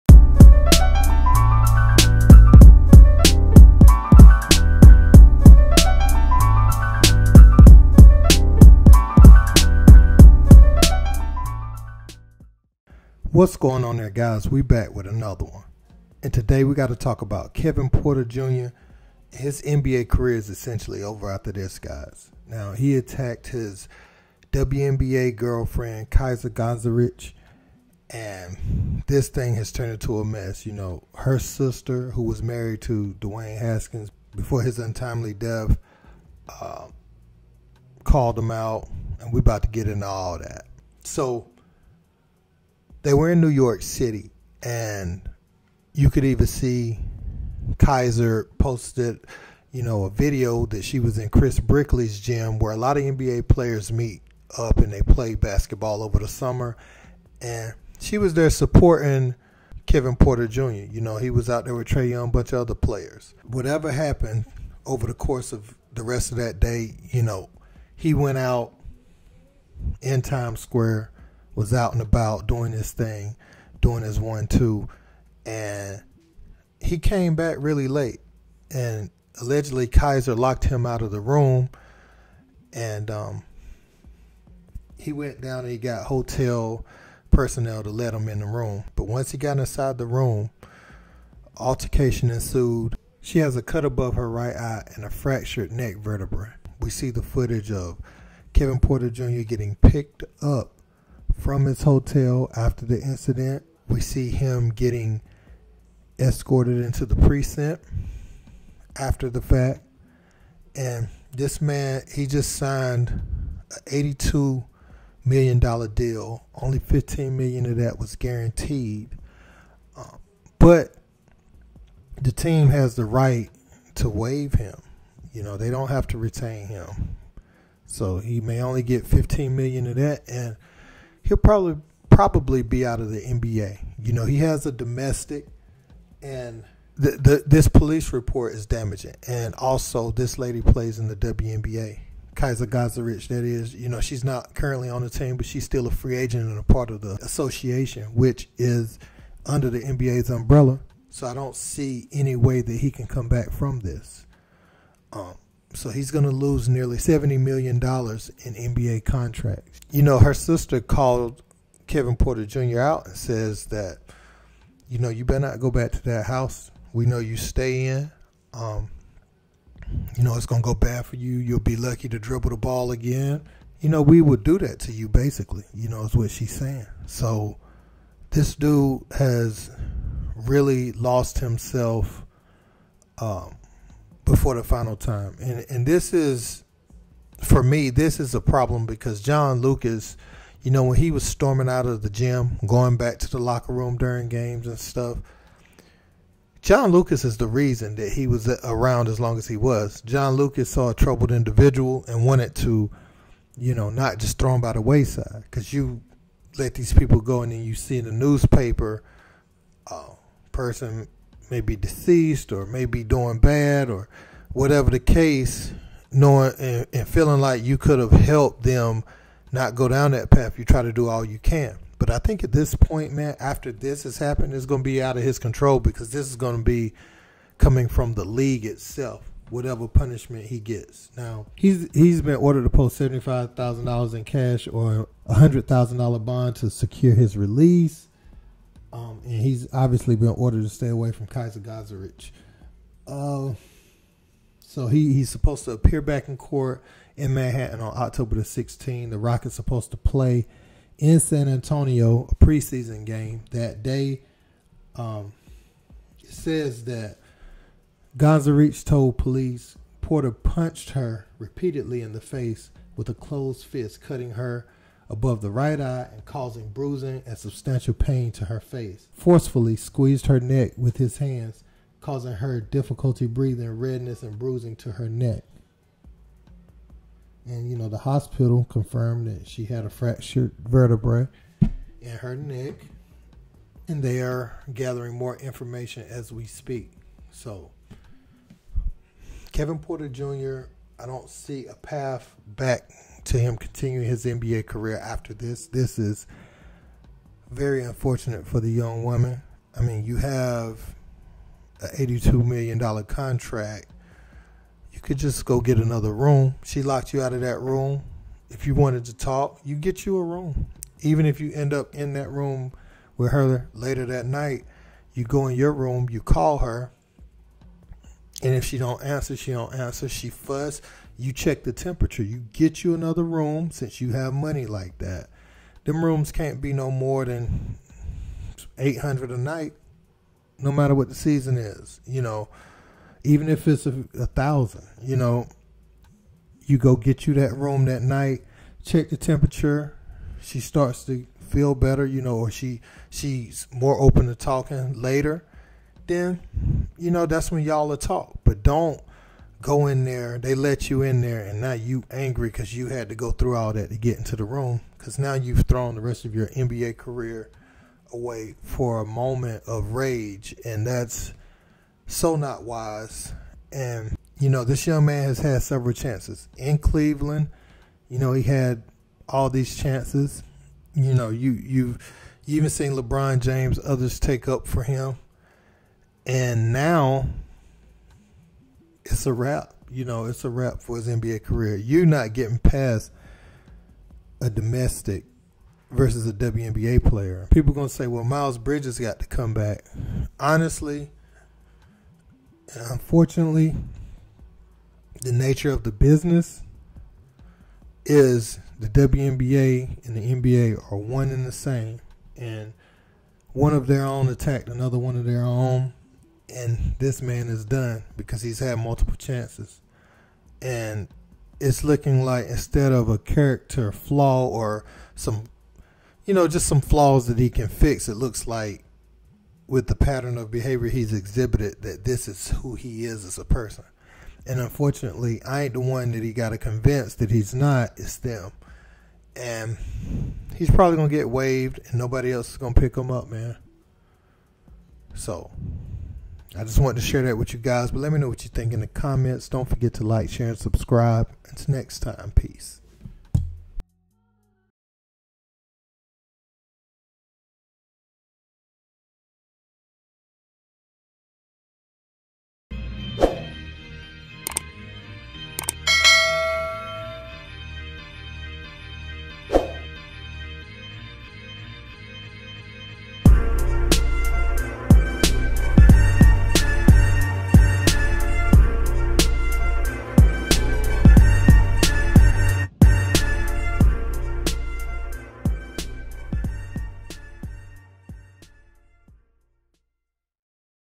Kevin Porter Jr Attacks WNBA Girlfriend Fractures Vertebrae Will Lose 70 Million Dollars From NBA!
Houston Rockets Guard Kevin Porter Jr attacked his WNBA Girlfriend Kysre Gondrezick who ended up with a cut above her right eye and a fractured Vertebrae. Her sister who is the wife of the late Dwayne Haskins said she would put him out of commission if she sees him ever again.